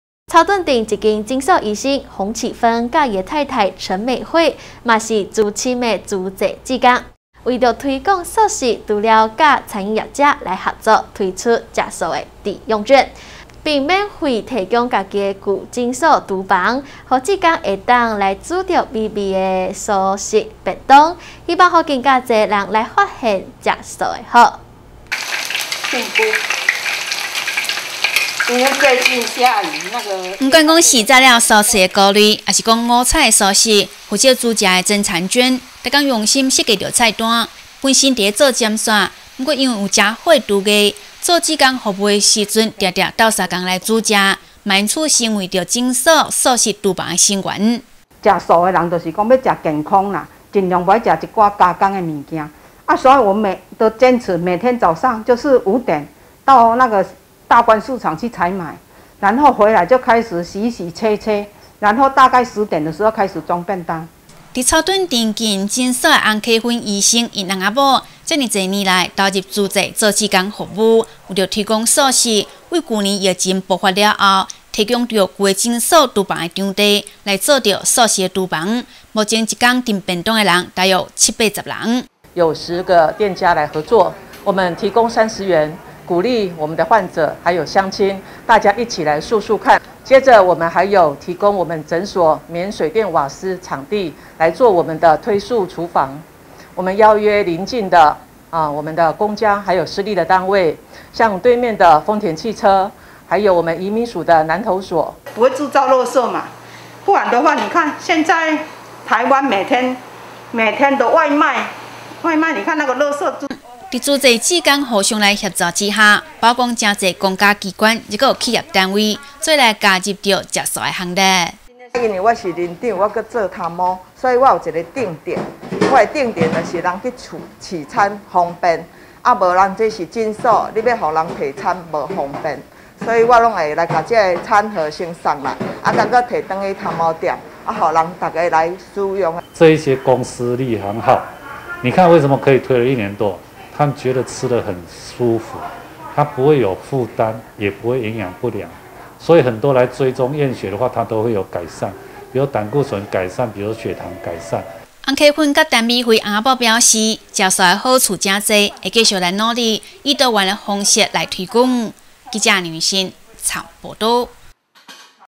慈濟志工推素食廚房 享用營養美味蔬食 南投新聞
草屯鎮診所醫生洪啟芬和太太陳美惠，也是資深慈濟志工，為了推廣素食，除了透過和餐飲業者合作，推吃素抵用券，並免費提供自己舊診所廚房，讓志工能烹煮美味的蔬食便當，希望讓更多人發現吃素的好。